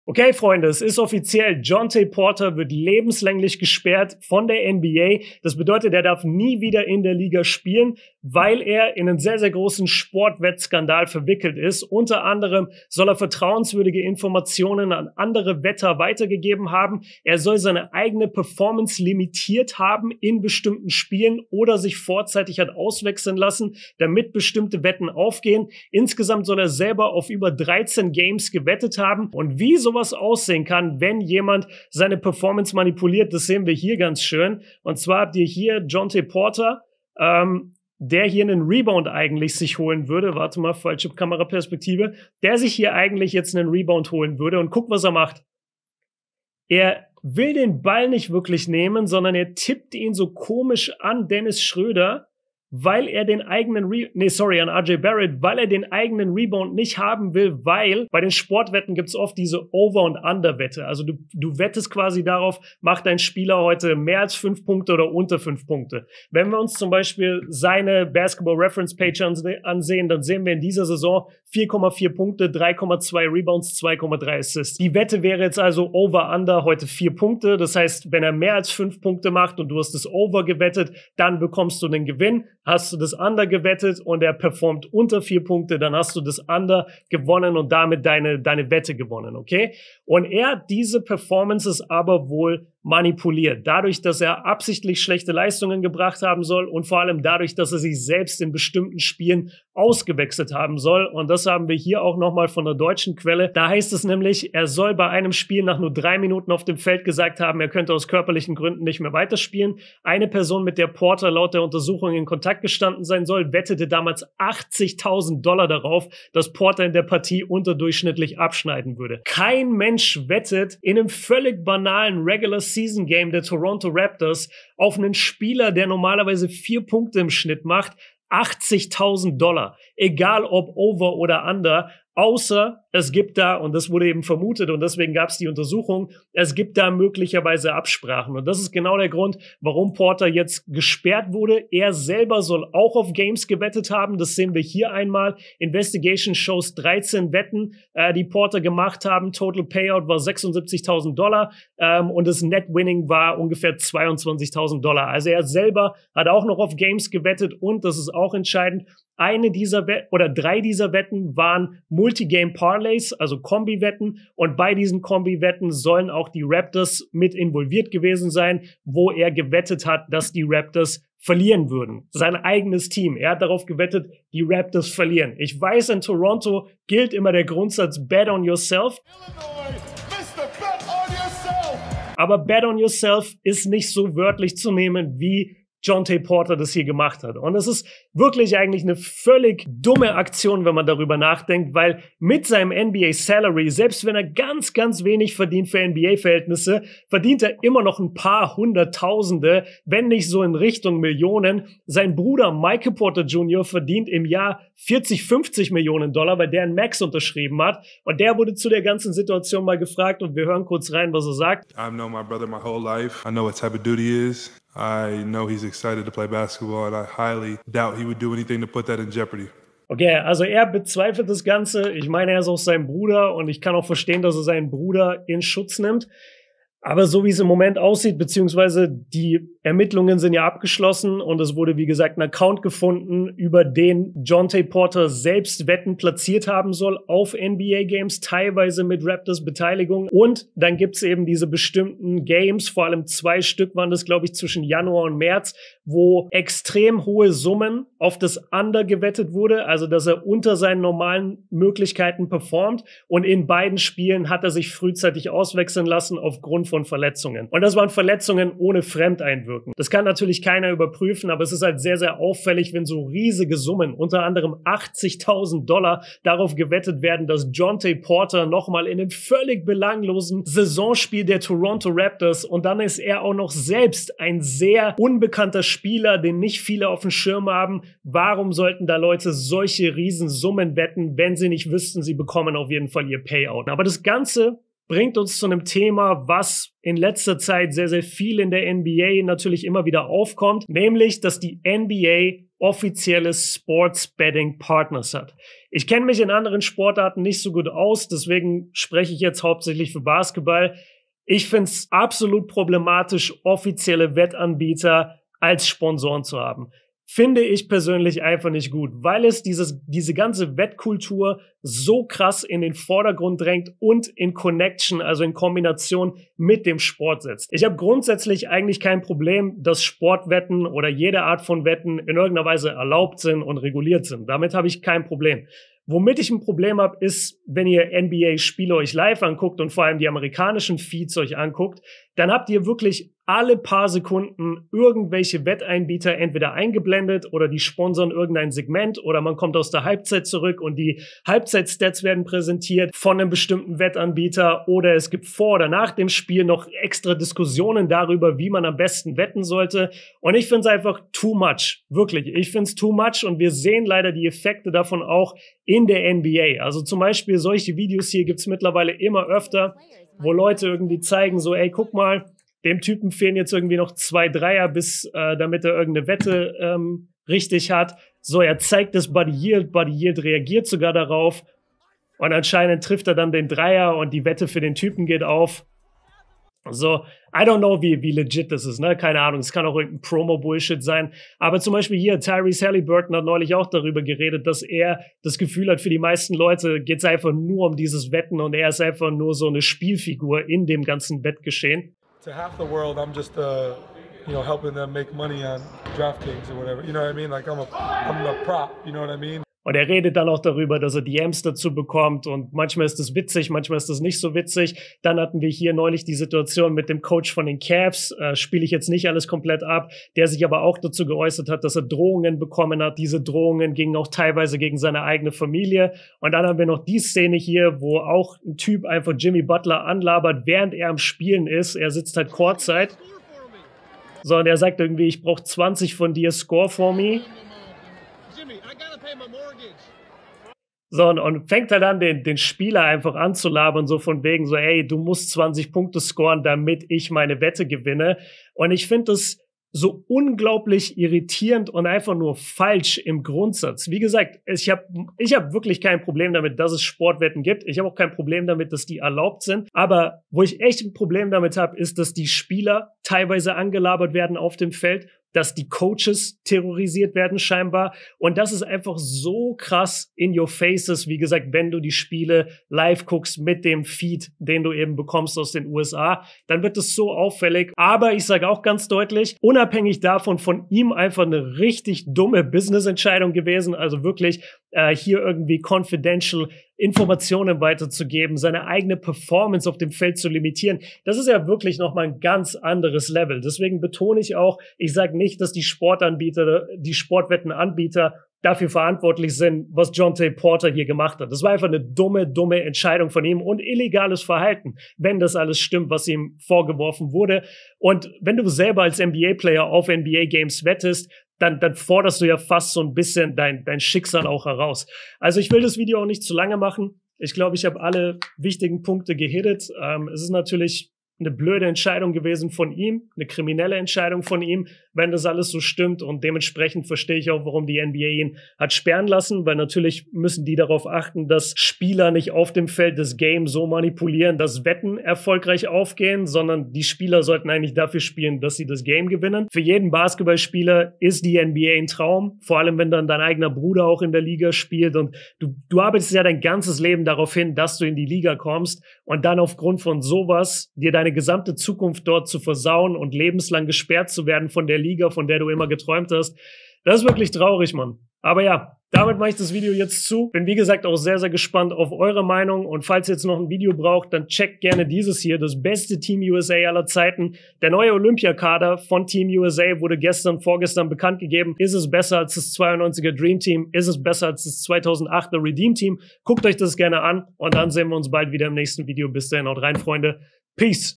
NBA Spieler LEBENSLANG Gesperrt wegen Sportwetten | BobeKjoern
Jontay Porter wurde wegen eines Wettskandals lebenslang von der NBA gesperrt. Der Bruder von Michael Porter jr. soll bis zu 13 Wetten selbst auf NBA Spiele platziert haben und vertrauenswürdige Informationen an andere Sportwetter weitergegeben haben. Außerdem soll er seinen eigene Leistung auf dem Spielfeld manipuliert haben um Wetten aufgehen zu lassen.